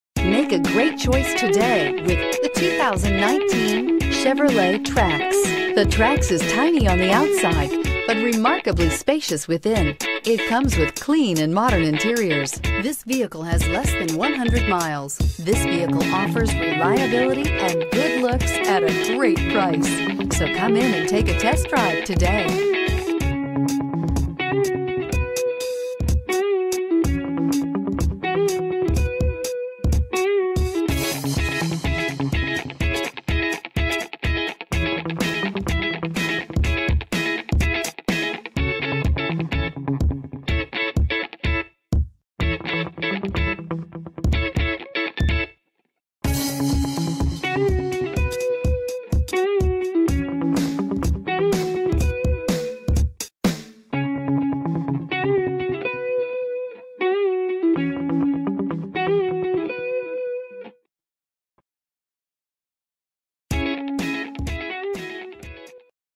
2019 Chevrolet Trax Rockford IL 19237
2019 Chevrolet Trax LS

Pick up this Gray Metallic 2019 Chevrolet Trax, available today at Lou Bachrodt Chevrolet Buick GMC. This could be the one you've been searching for.